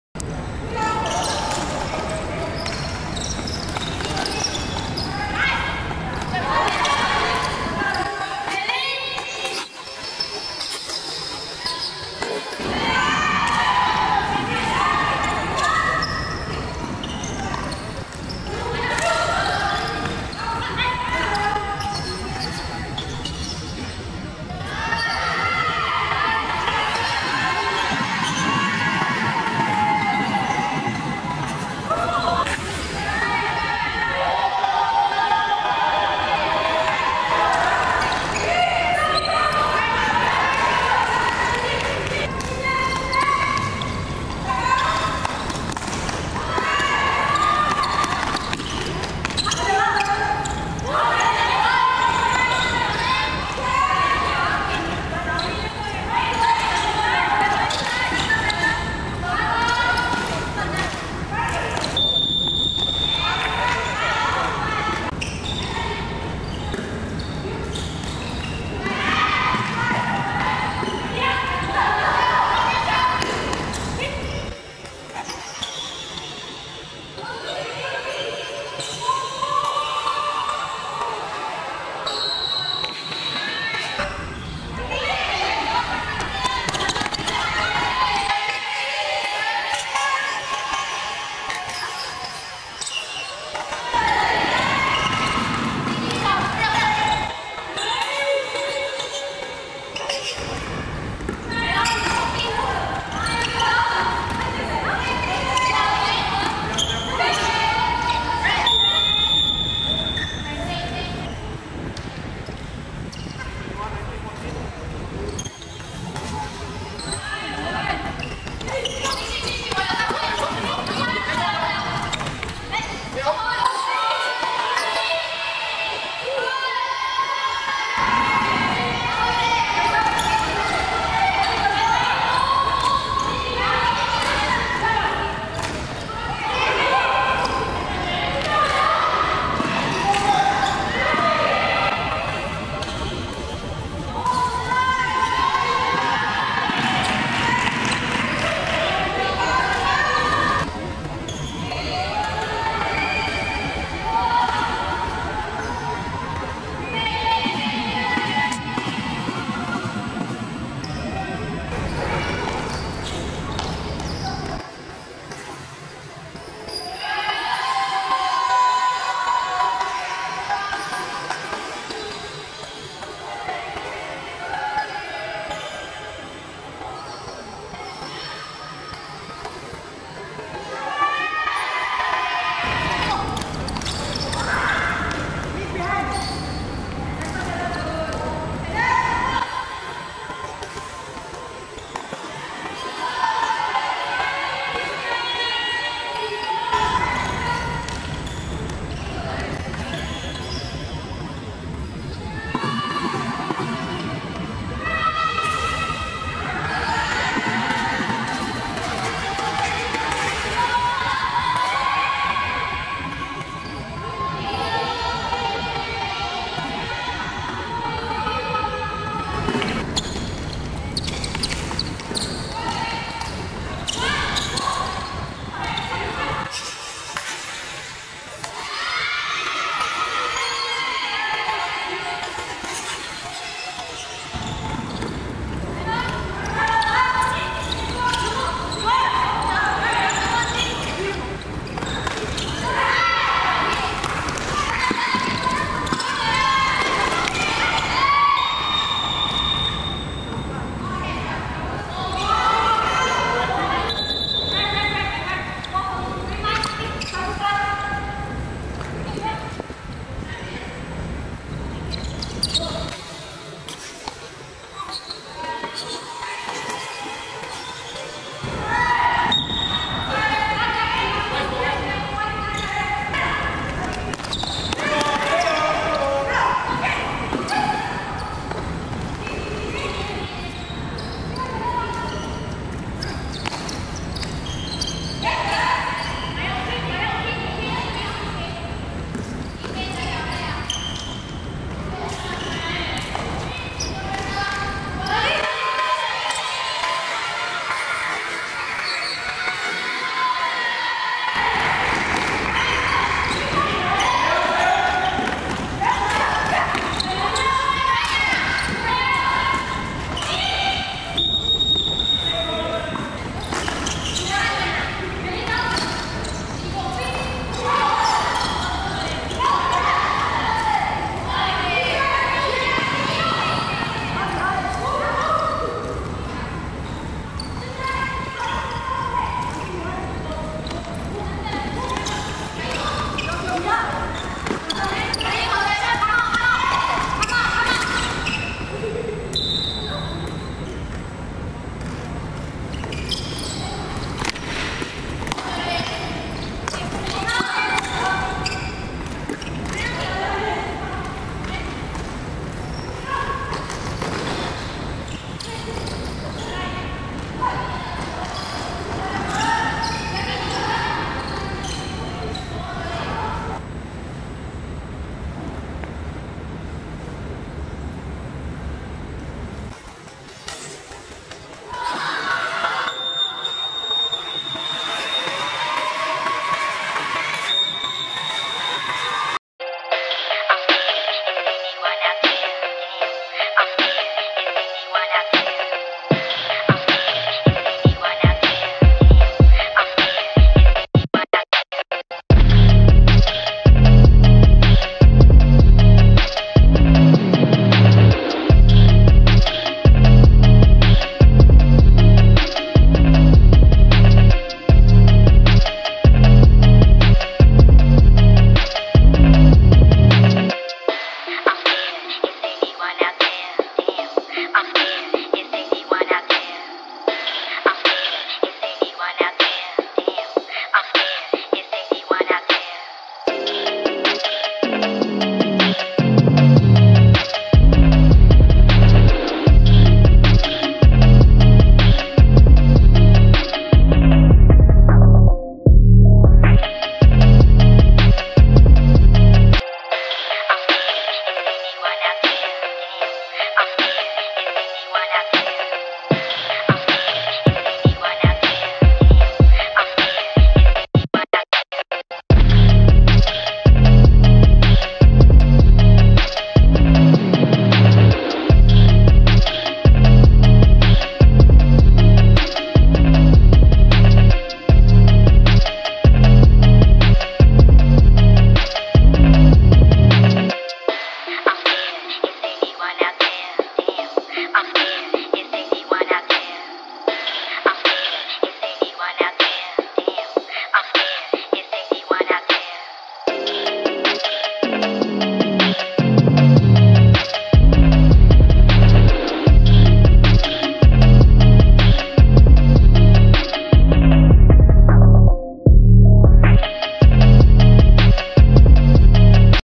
Cripz vs Titans - Games Recap I "Singapore Floorball Women's Premiere League 23/24"
@theberdanfam1197 Cripz vs Titans - Singapore Floorball Women's Premiere League 23/24

Cripz vs Titans - 31 Aug. 2024
Final Score: 3 - 2
MOTM: #59 Kate